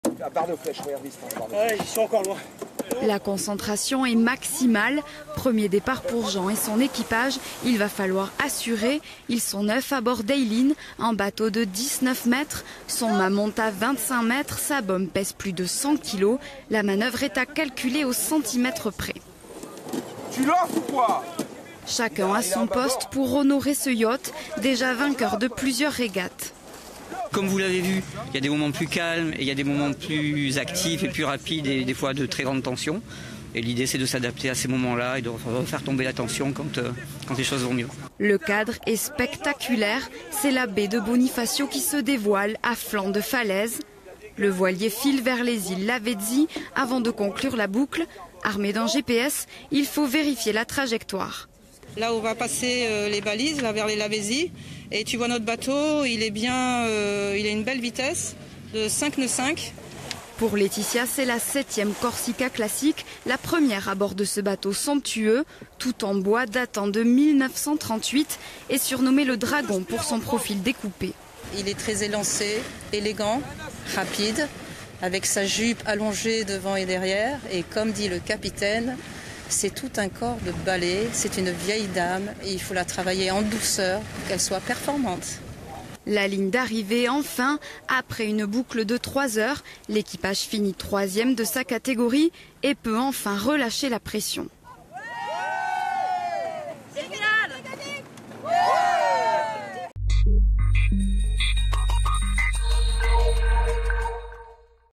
Bonifacio : embarquement sur le « Eileen » pour le départ de la 10e édition de la Corsica-Classic
Le départ de la 10e édition de la Corsica-Classic a été lancé à Bonifacio ce lundi 26 août. Il s’agit de la première étape avec une régate au pied des falaises avant plusieurs étapes par l'est jusqu'à Saint-Florent. Embarquement sur un des yachts en compétition.

Intervenants - Eric Adjadj, Equipier ; Laetitia Morand-Monteil, Equipière.
Equipe - Cécile Bonte ; Ophélie Perroux ; Frédéric Guichard.